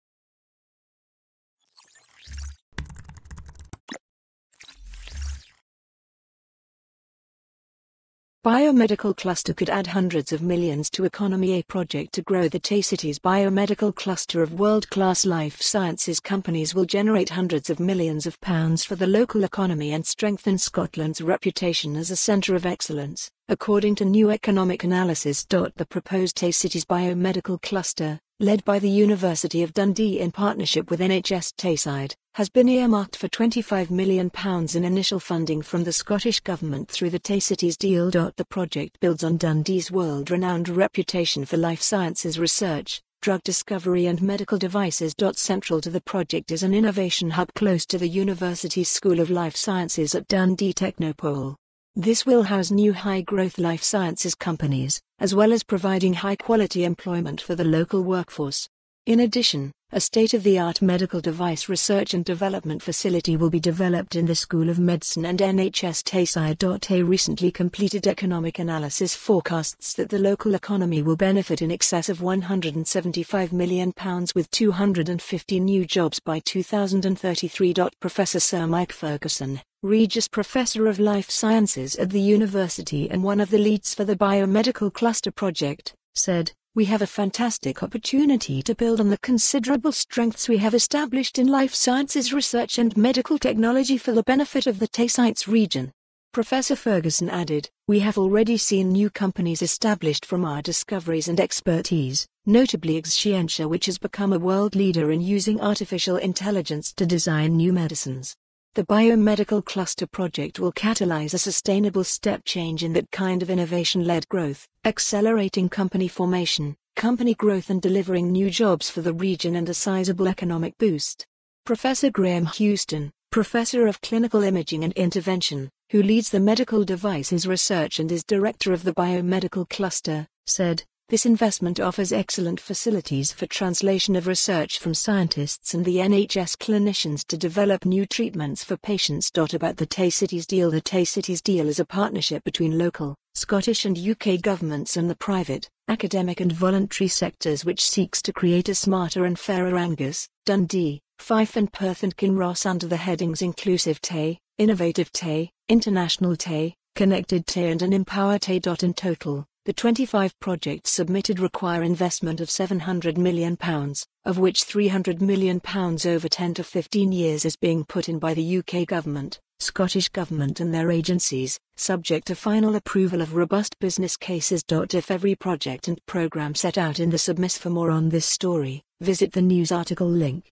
Biomedical Cluster could add hundreds of millions to economy
Reported today on Deadline News

A project to grow the Tay Cities Biomedical cluster of world-class life sciences companies will generate hundreds of millions of pounds for the local economy and strengthen Scotland's reputation as a centre of excellence, according to new economic analysis.The proposed Tay Cities Biomedical Cluster, led by the University of Dundee in partnership with NHS Tayside, has been earmarked for £25million in initial funding from the Scottish Government through the Tay Cities Deal.The project builds on Dundee's world-renowned reputation for life sciences research, drug discovery and medical devices.Central to the project is an Innovation Hub close to the University's School of Life Sciences at Dundee Technopole. This will house new high-growth Life Sciences companies, as well as providing high quality employment for the local workforce. In addition, a state of the art medical device research and development facility will be developed in the School of Medicine and NHS Tayside.A recently completed economic analysis forecasts that the local economy will benefit in excess of £175million with 250 new jobs by 2033.Professor Sir Mike Ferguson, Regius Professor of Life Sciences at the University and one of the leads for the Biomedical Cluster project, said, "We have a fantastic opportunity to build on the considerable strengths we have established in life sciences research and medical technology for the benefit of the Tay Cites Region. Professor Ferguson added, "We have already seen new companies established from our discoveries and expertise, notably Exscientia which has become a world leader in using artificial intelligence to design new medicines. The BioMedical Cluster project will catalyse a sustainable step change in tha